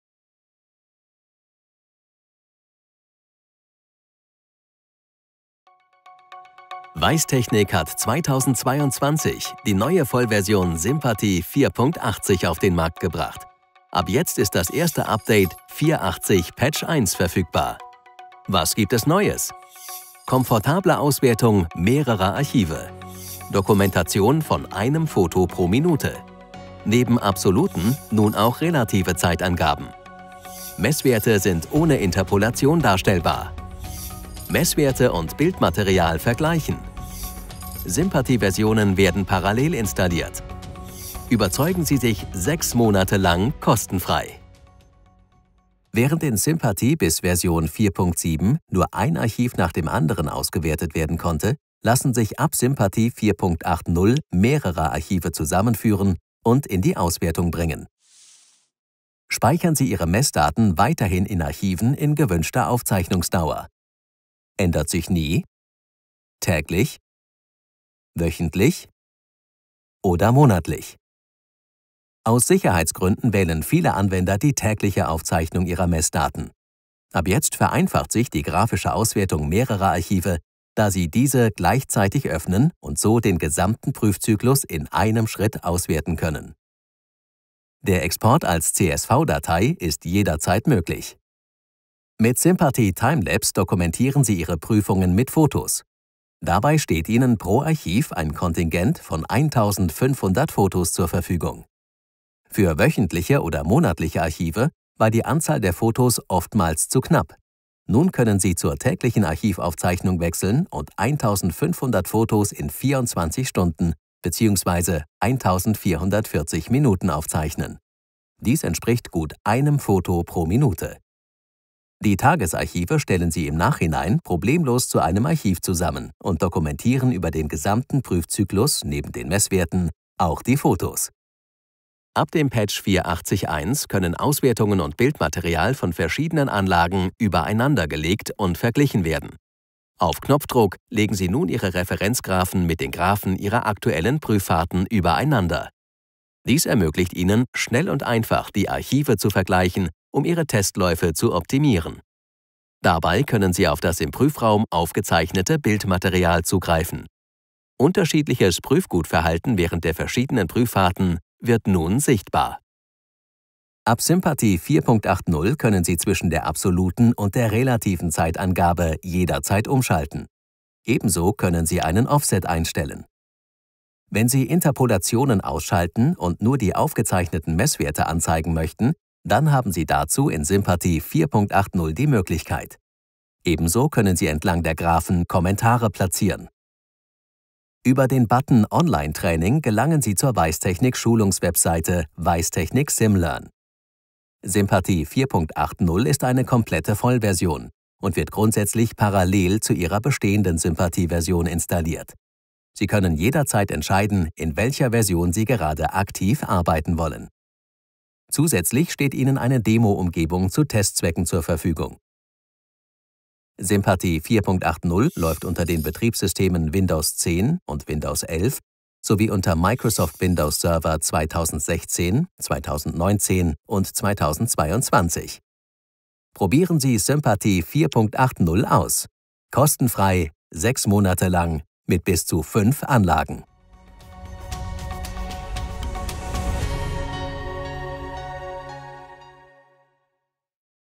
S!MPATI 4.80.1 - das sind die Highlights der neuesten Version
S!MPATI 4.80.1 ist die weisstechnik software zur Steuerung und Auswertung von Prüfläufen in der Umweltsimulation. Klimadaten der Klimaprüfschränke können gemeinsam mit Testdaten der Prüflinge sowie Bildmaterial aus den Prüfkammern komfortabel aufbereitet werden. Highlights der Version sind die Auswertung mehrerer Archive, die Dokumentation von einem Foto pro Minute, neben den absoluten nun auch die relative Zeitangaben, die Darstellung der Messwerte ohne Interpolation sowie die Vergleichbarkeit von Messwerten und Bildmaterial.